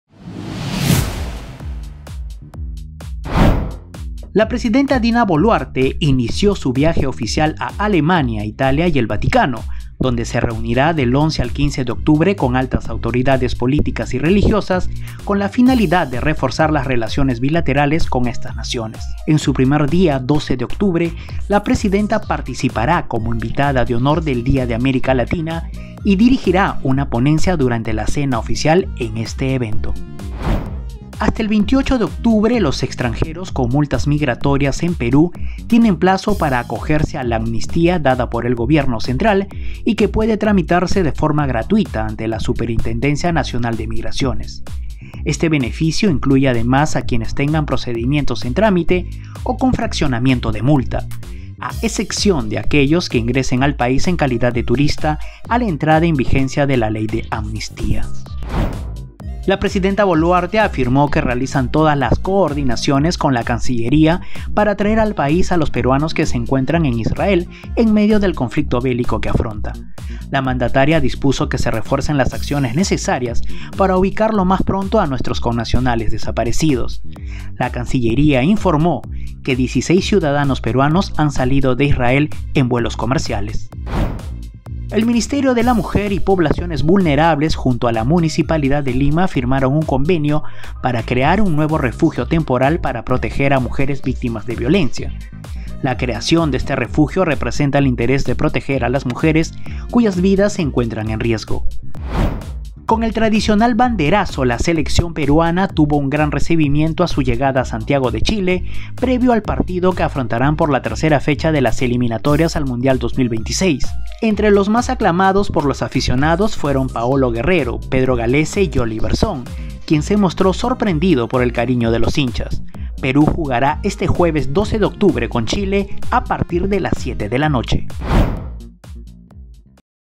Las 5 del día: Presidenta Boluarte inicia viaje oficial a Alemania, Italia y el Vaticano
1.- Presidenta Boluarte inicia viaje oficial a Alemania, Italia y el Vaticano

2.- 28 de octubre vence plazo de amnistía para extranjeros con multas migratorias

3.- Peruanos varados en Israel serán evacuados

4.- El Ministerio de la Mujer y Poblaciones Vulnerables junto a la

5.- Selección peruana tuvo gran recibimiento en Chile